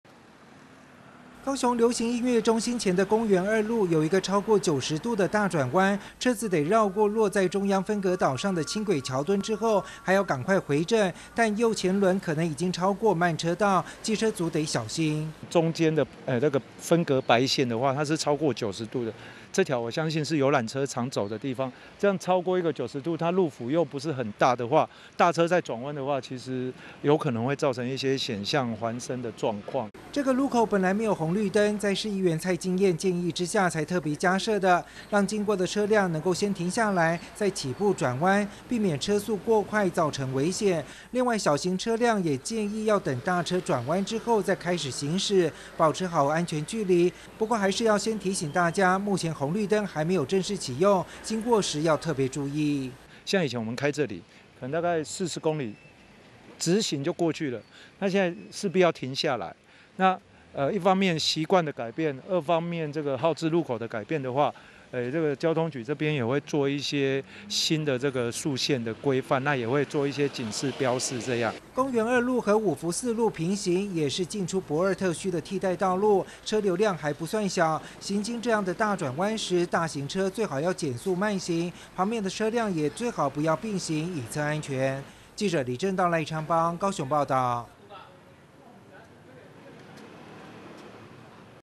高雄輕軌｜公園二路大轉彎 避免併行以策安全
公園二路大轉彎 避免併行以策安全

高雄流行音樂中心前的道路規劃，受限於輕軌橋橔的影響，在公園二路口，形成一個超過九十度的大轉彎；市議員蔡金晏建議市府，在路口設置紅綠燈，讓車輛能放慢度，當大型車輛在轉彎時，旁邊的車輛也能保持距離，避免發生危險。